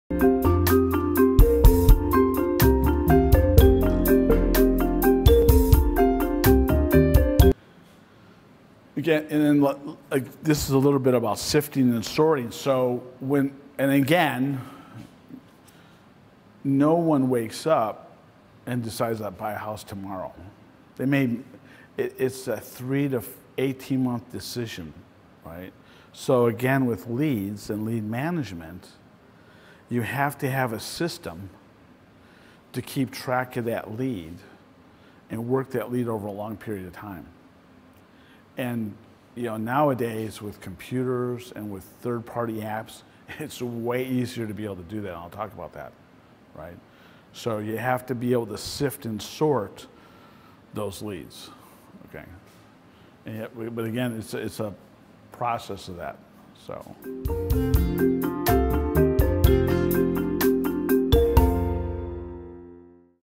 What’s the Life Cycle of a Lead?
Nobody wakes up and decides to buy a house tomorrow. Buying a house is a three- to 18-month decision. You have to have a lead management system to keep track of that lead over time. With computers and third-party apps, it’s way easier to do these days. To hear my full message, watch this short video.

Frank Valente l Coach, Speaker, Entrepreneur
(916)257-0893
CoachWithFrank.com
915 Highland Point Dr. Suite 210